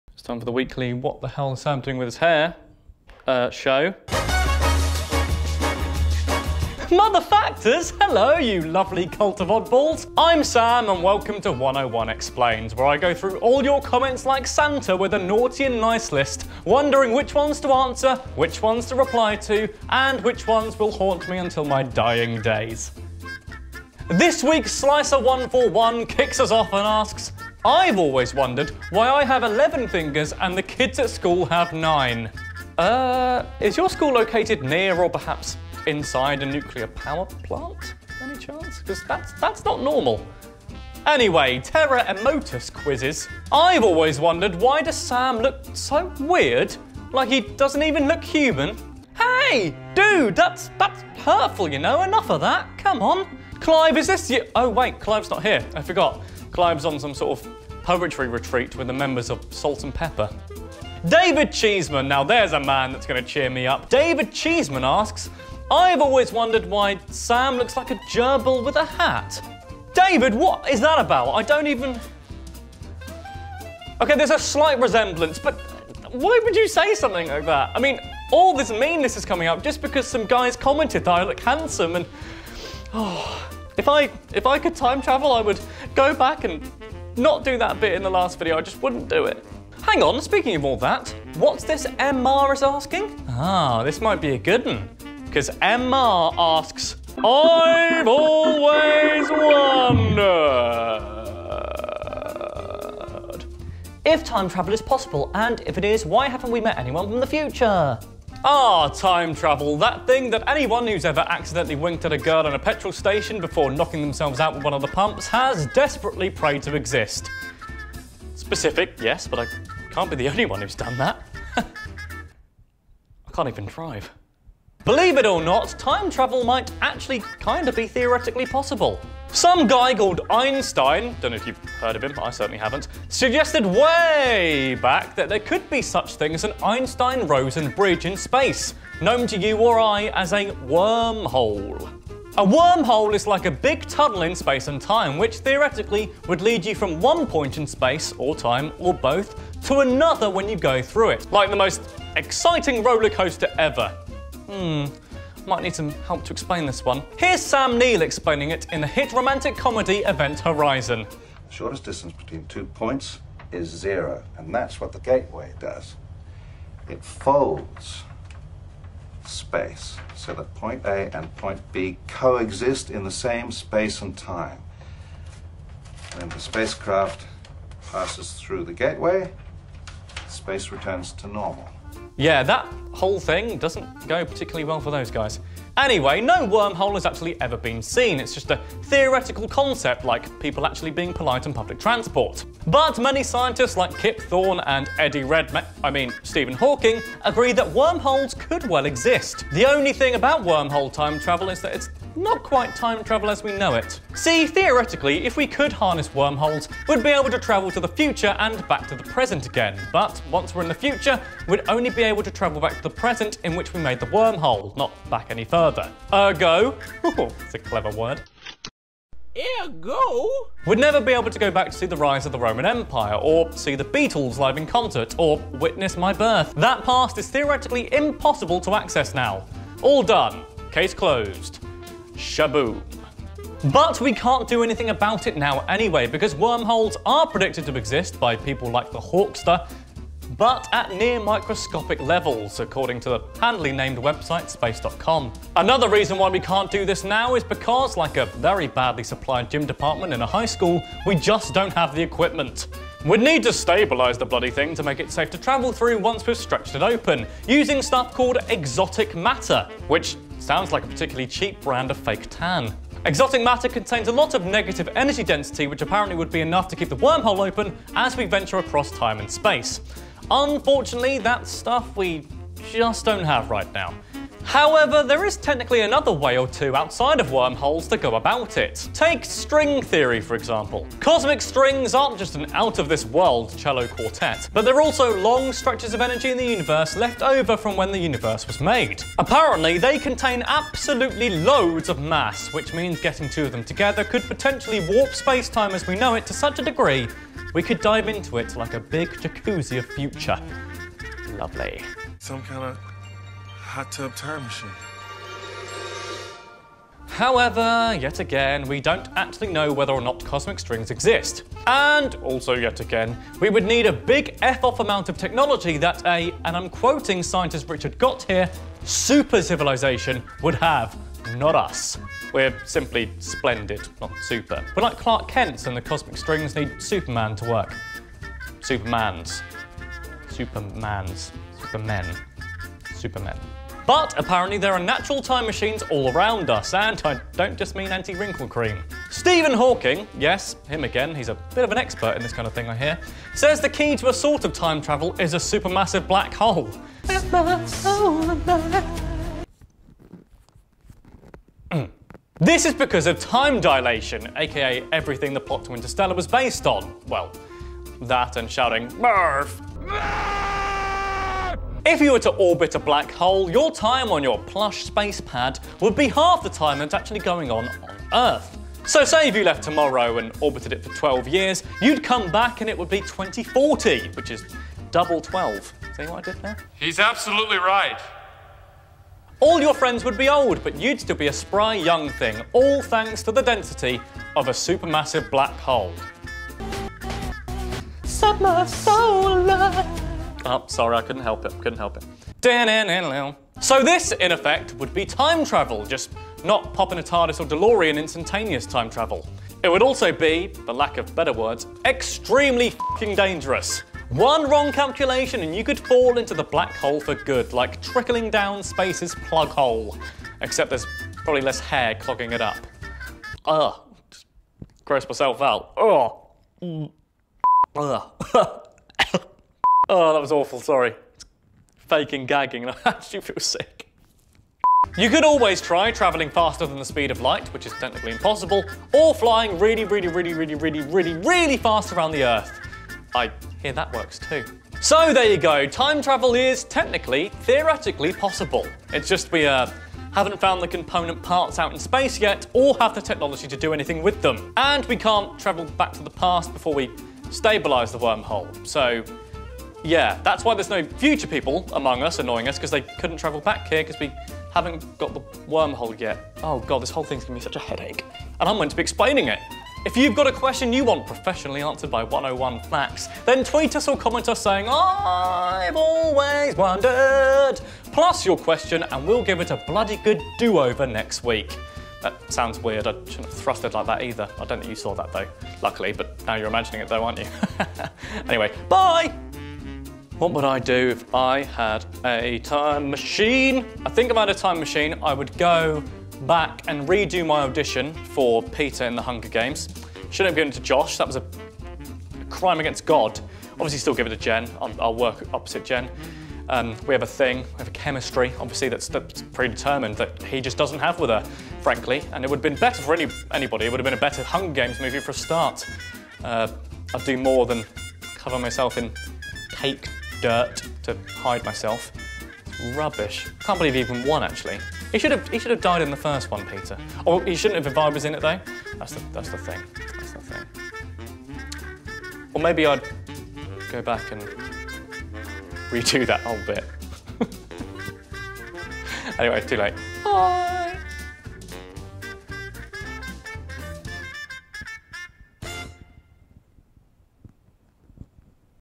Is Time Travel Possible? 101 Facts Explains....
So the guys in the movies and on TV have done it, so why can't we??!! Well it turns out we can. . . . but we can't at the same time. I'm going to explain why this is, in this episode of 101 Explains